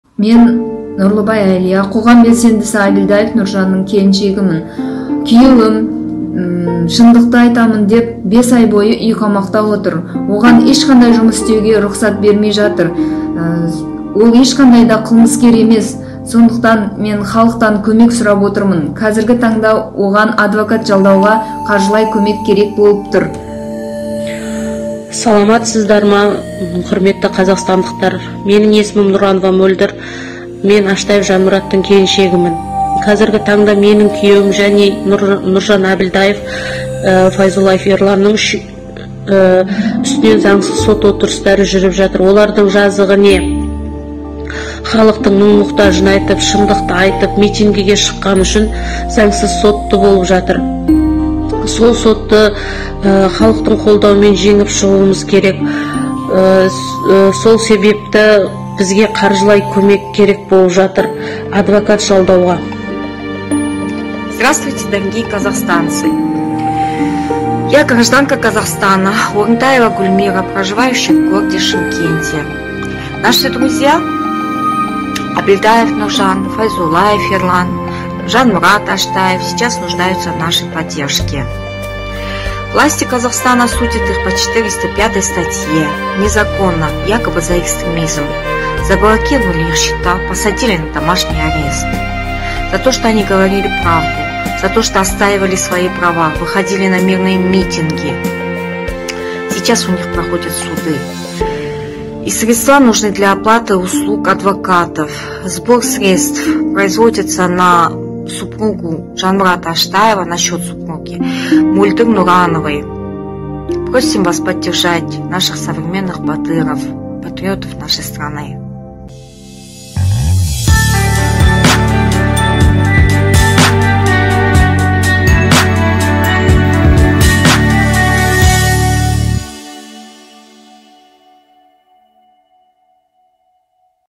ФИНАНСОВАЯ ПОМОЩЬ АКТИВИСТАМ ШЫМКЕНТА
Гражданские активисты стоят на переднем фланге борьбы с диктаторским режимом. Они смело выступают против беззакония и ведут к нашей общей цели — демократии и процветанию Казахстана. Они — рупор и авангард протеста.
Назарбаев понимает, что активное общество лишит его власти над страной, народ Казахстана обретёт свободу, а сам диктатор потеряет власть и предстанет перед судом. Поэтому он и старается уничтожить дух протеста.

Сейчас под следствием находятся три активиста из Шымкента:

- Нуржан Абильдаев (3 несовершеннолетних детей, счета заблокированы)
- Жанмурат Аштаев (5 несовершеннолетних детей, счета заблокированы)
- Ерлан Файзуллаев (7 несовершеннолетних детей, счета заблокированы)

Мы не должны позволить режиму сломать их жизни!
Даже если вы по каким-то причинам не можете принимать активное участие в протестном движении, у многих из вас есть возможность финансово поддержать активистов и помочь им материально. Тысяча тенге — очень скромная сумма, но при этом вы будете знать, что благодаря вашей помощи, активисты выстоят и продолжат борьбу.

Сбор средств осуществляется на счёт супруги Жанмурата Аштаева:

MOLDIR NURANOVA
Каспий голд:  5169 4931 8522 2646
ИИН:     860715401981

АЛГА КАЗАХСТАН!

1. Загрузите анонимный мессенджер Telegram на смартфон или компьютер с официального сайта
    2. Зарегистрируйте профиль с помощью мобильного номера